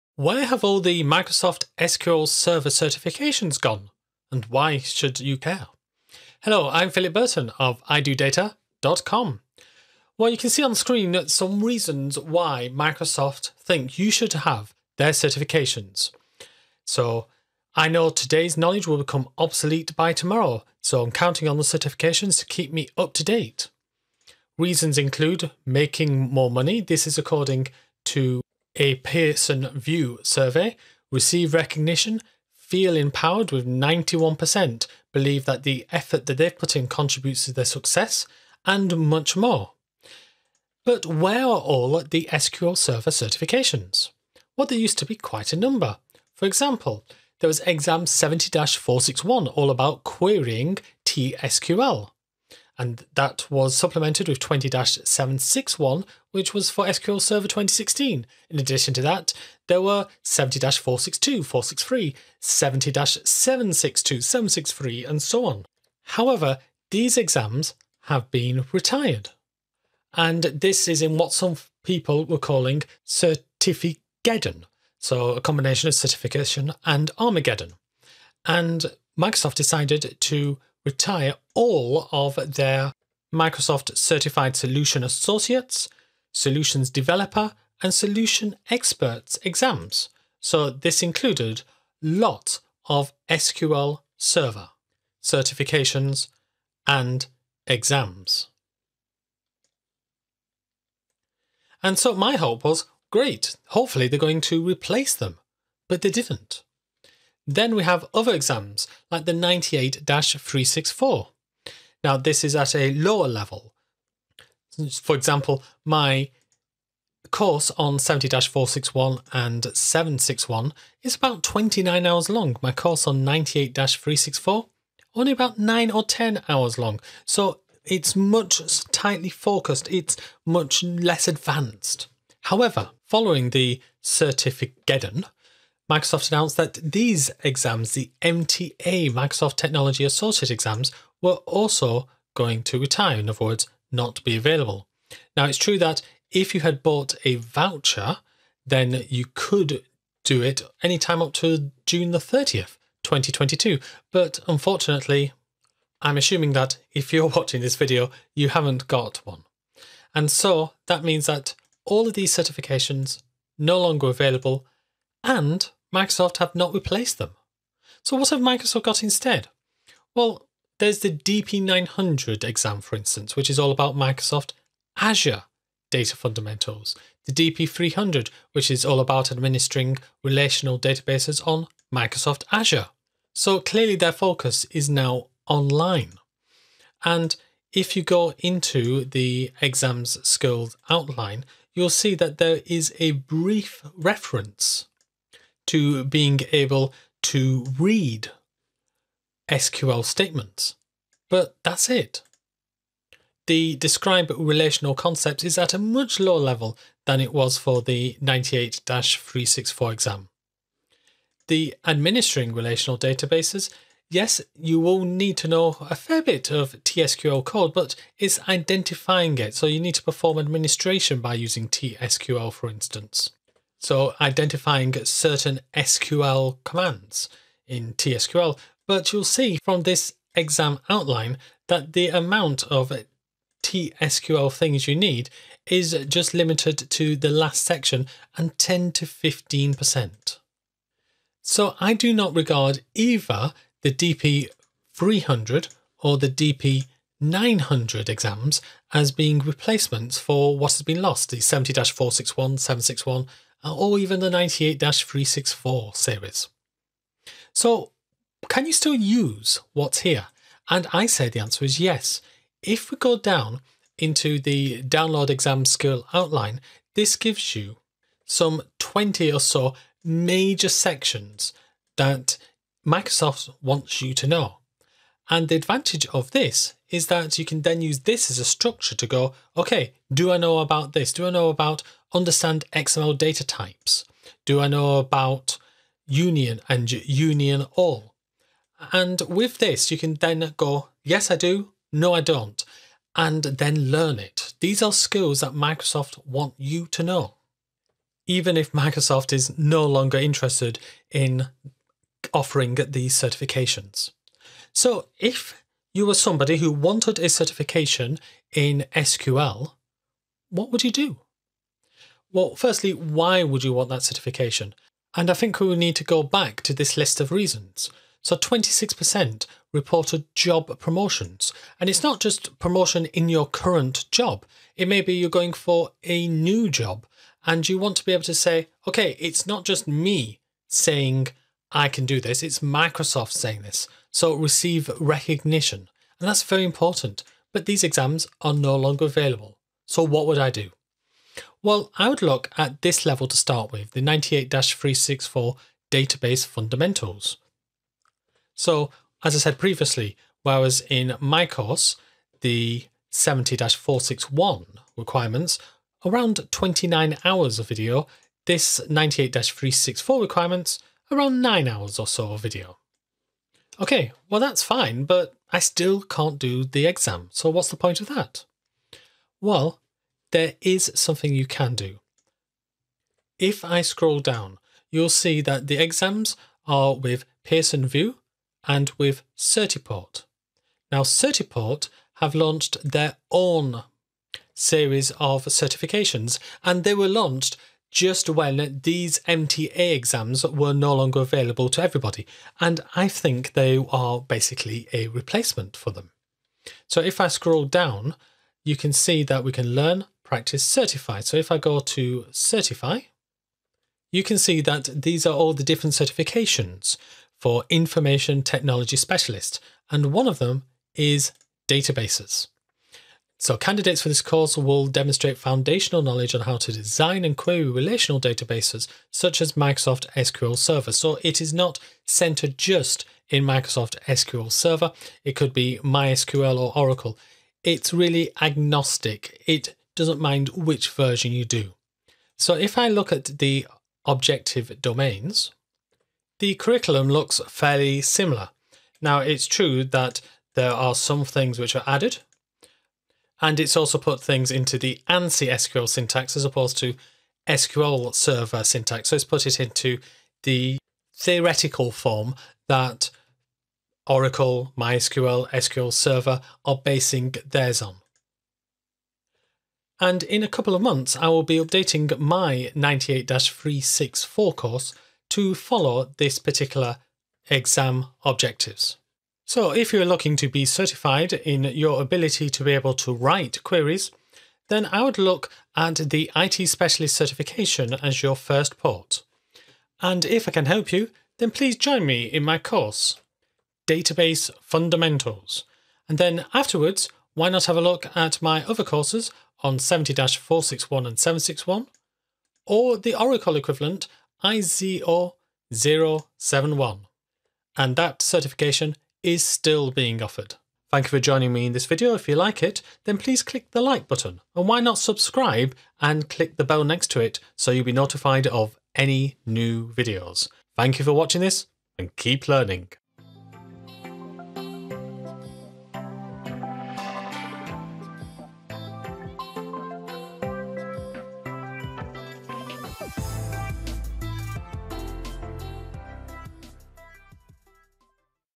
Can I do a certification in SQL Server?
There used to be many certifications in SQL Server. But where have they all gone?
My SQL Server Udemy courses are:
There used to be many certifications in SQL Server, such as 70-461, 70-761 and 98-364. However, in early 2021 they all disappeared.
In this video, we'll have a look at the reasons why they disappeared, and a current alternative